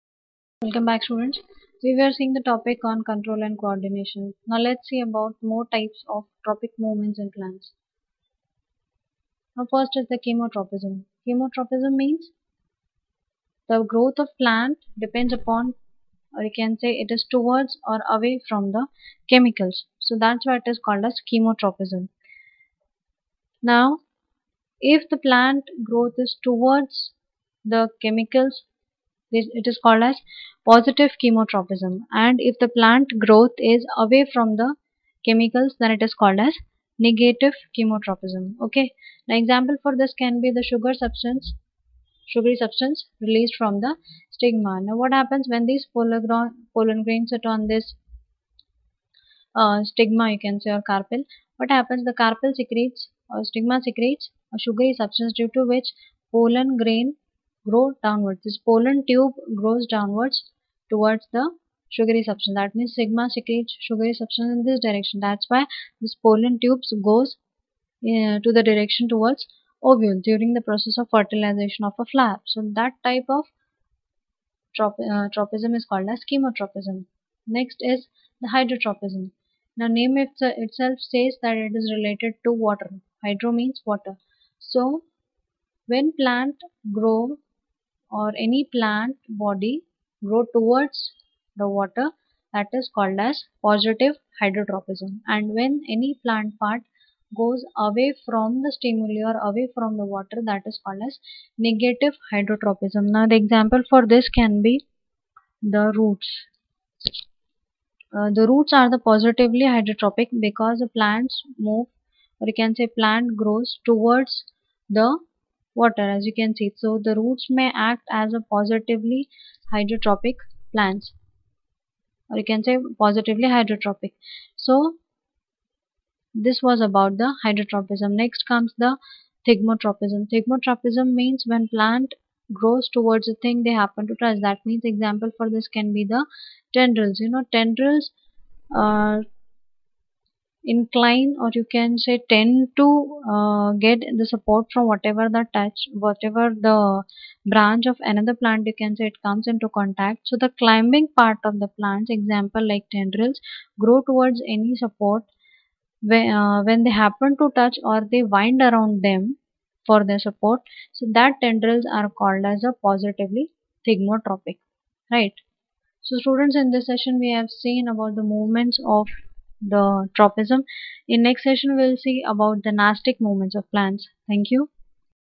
Control and Coordination In Plants Part 3
Control is the power of restrain and regulation by which something can be started,slowed down or stopped. Co-ordination : the working together of various agents of the body of an organisms in a proper manner to produce appropriate reaction to a stimulus is called coordination
Coordination is the ability to use different parts of the plant together, smoothly and efficiently. In plants, coordination is due to the result of a chemical system, wherein plant hormones or phytohormones have a major role.